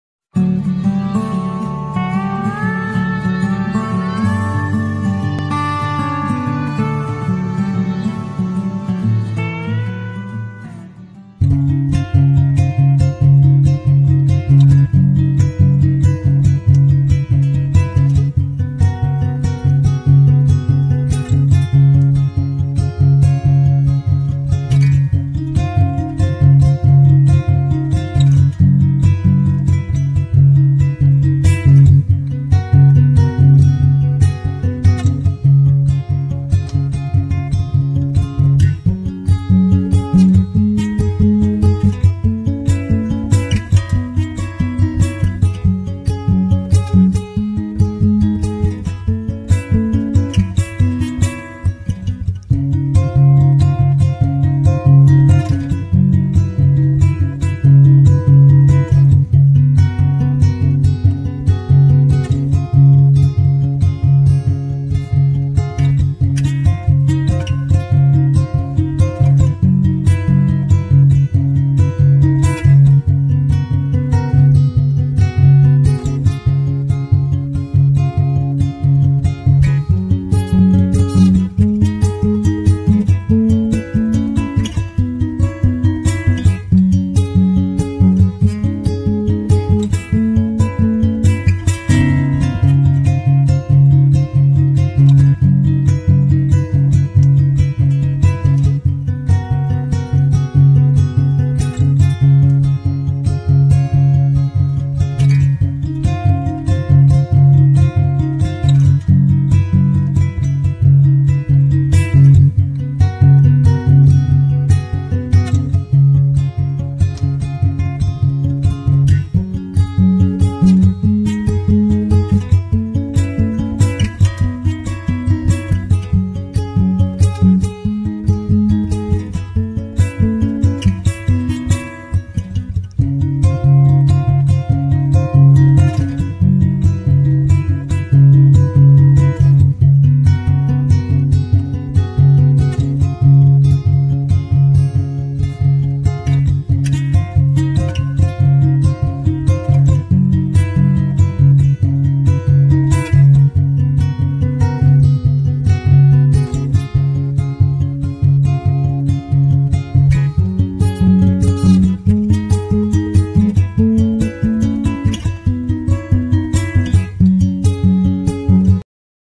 Narkel'er bora / coconut pakoda / Bengali recipe
This coconut fritters are a great way to take your bhaat dal bhaja game to the next level. this is also tasty snack.